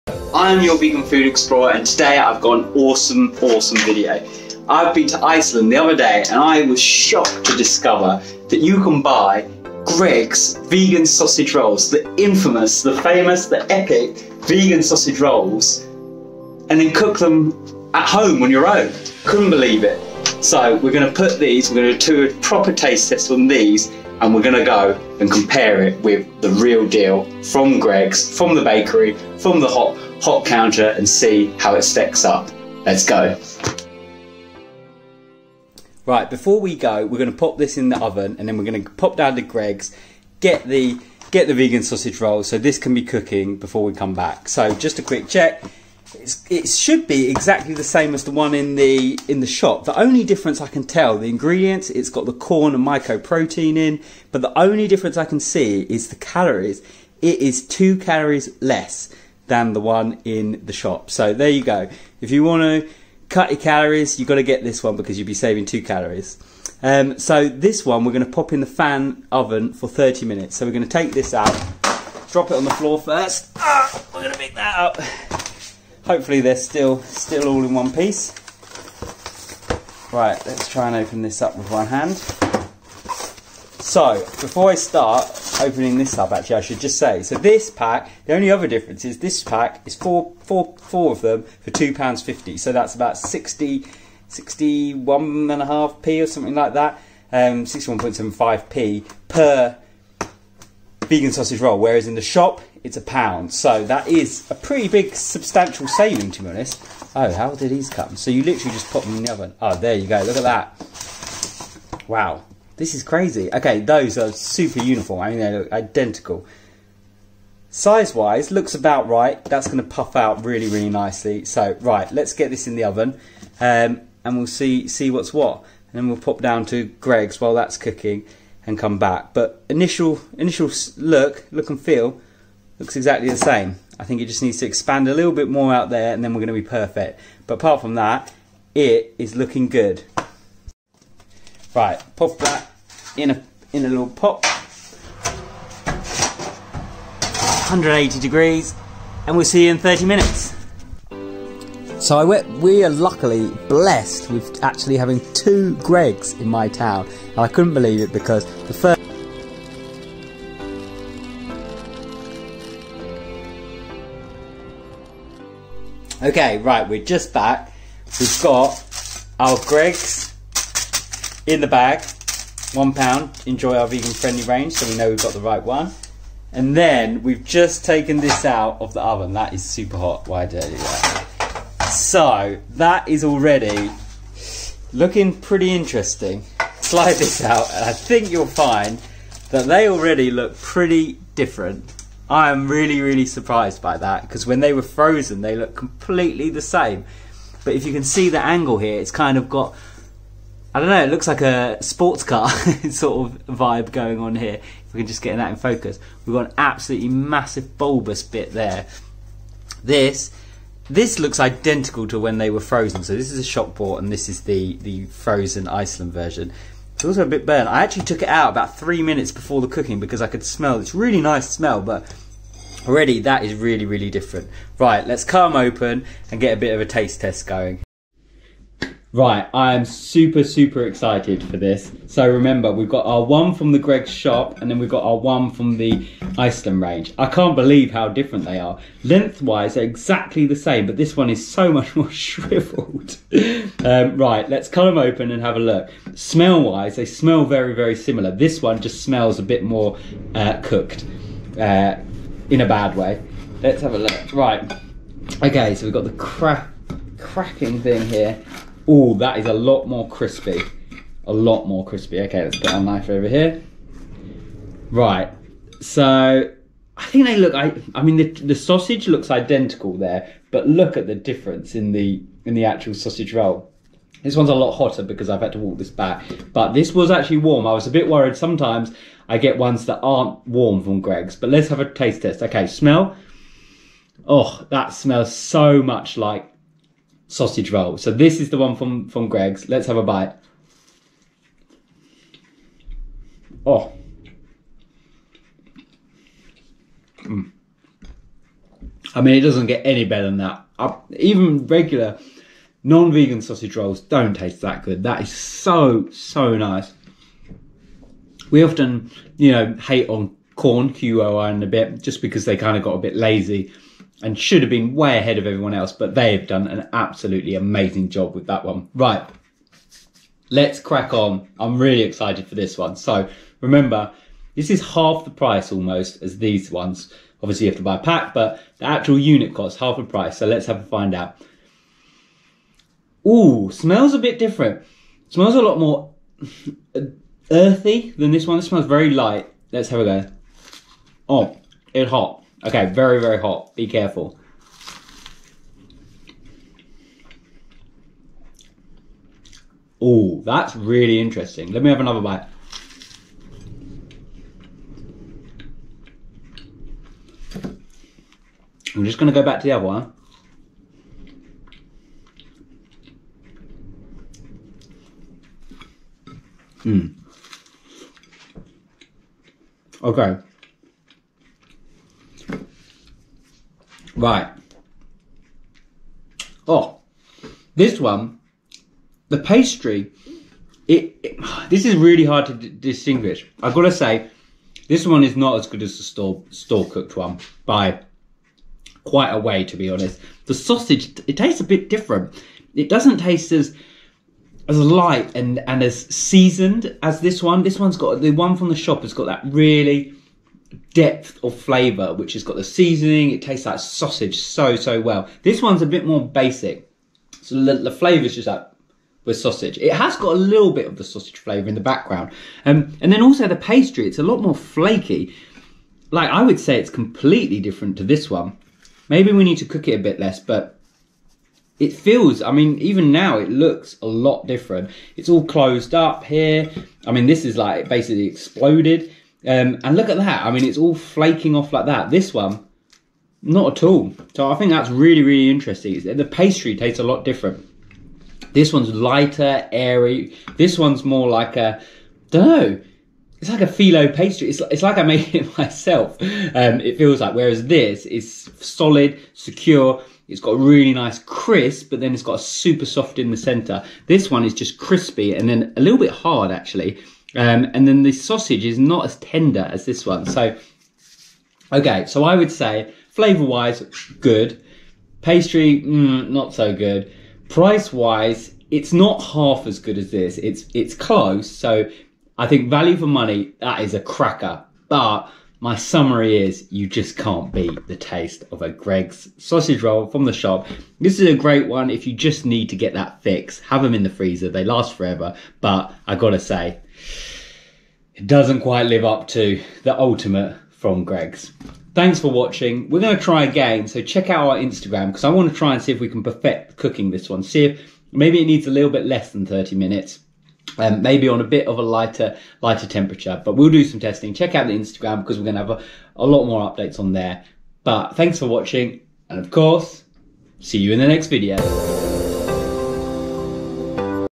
We Compare Iceland Greggs Oven Cook Vegan Sausage Roll To The Real Thing
❄️❄️ Greggs Vegan Sausage Rolls From Iceland ❄️❄️
HOW IS THIS A THING AND I'M JUST DISCOVERING IT?!?!?
I normally avoid @icelandfoods as I can't stand that they have no self checkouts in my local store and that they normally have 1 super slow checkout person for a whole store - it's 2021!!??
BUT you can't get past the fact that they have some crazy crazy deals and some really good vegan food offerings too!
I was on a mission for vegan magnums, but the first freezer cabinet I walked past was a whole @greggs_official branded one!! I dared to dream...they had the meat sausage rolls. and they actually had the vegan versions to cook at home! I honestly was trying to find someone to tell in the shop I was that amazed 😆😆
You get 4 in a pack for £2.50 which is an absolute bargain as in-store they are £1, so almost half the price per roll!
I know the real question, how do these fair against the store bought version??